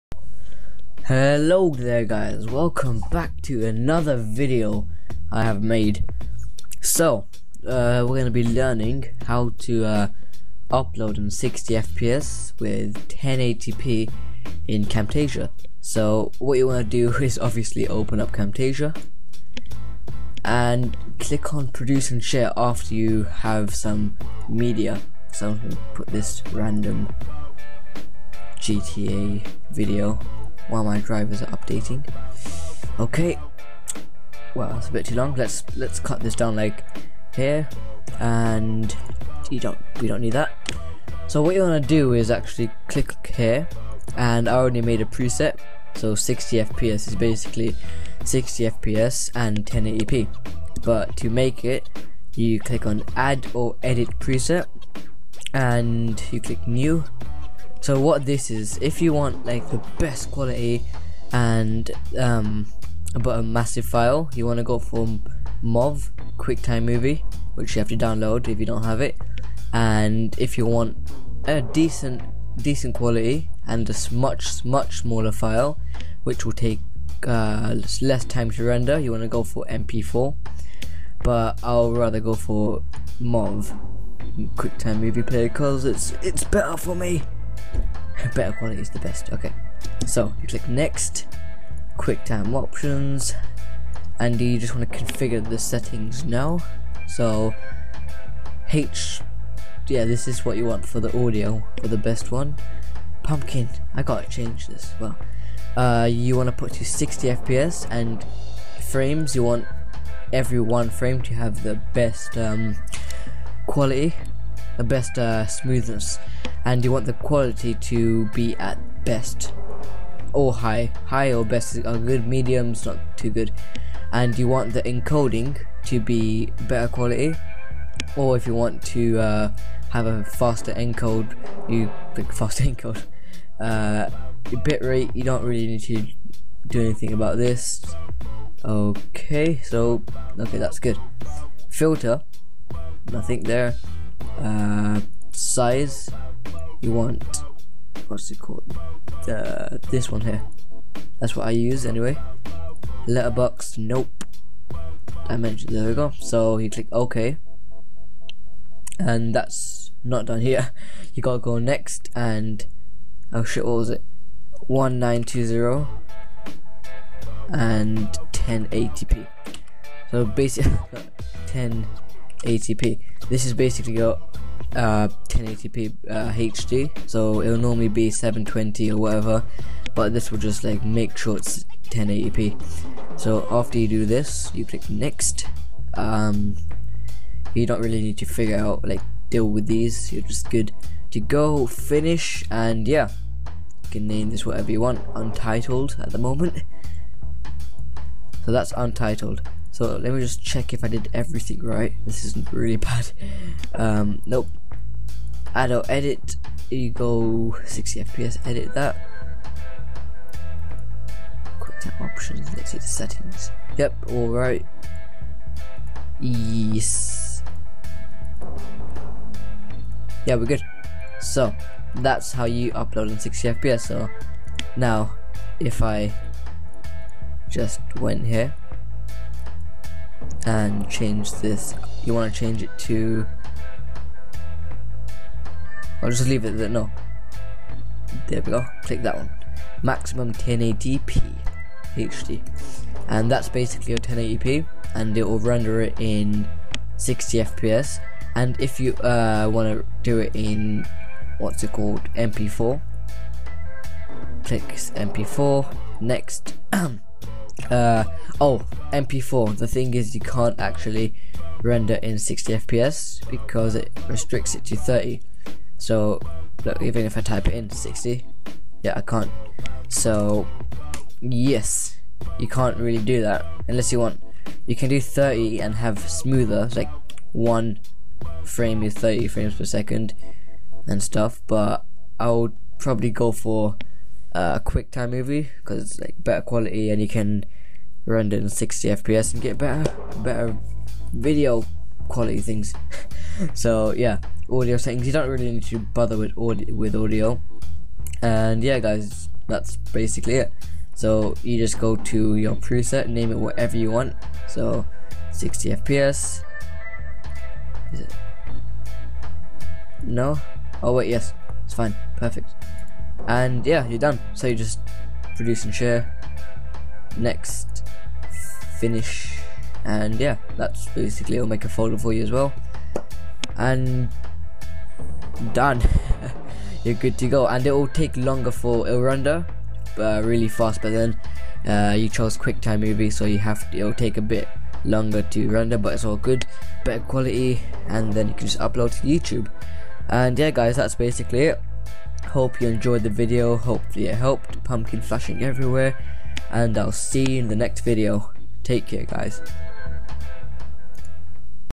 How To: Render/Upload In 1080p & 60FPS Using Camtasia
Hey there people, hope this video can help out some of you, and hopefully you dont want to kill me because of all the blabbering haha, enjoy the video guys!

And here are some downloads for Ensharpen and Quicktime in case you dont have the .mov option: